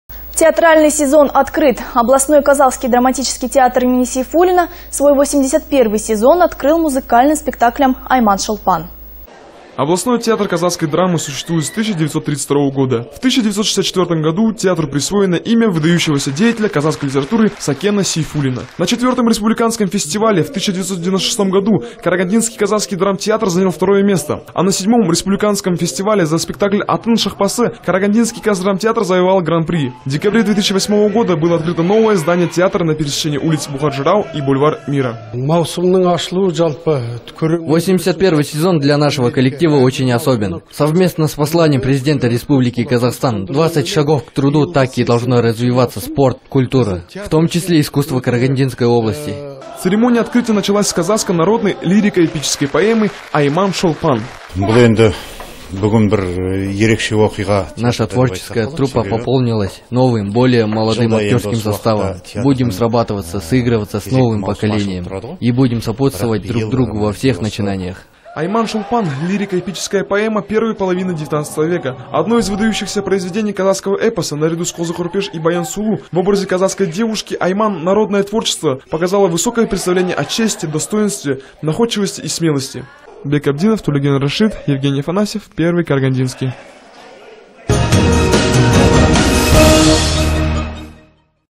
10_09_12_театр
Театральный сезон открыт. Областной казахский драматическом театр имени Сейфулина свой 81 сезон открыл  музыкальным спектаклем «Айман шолпан».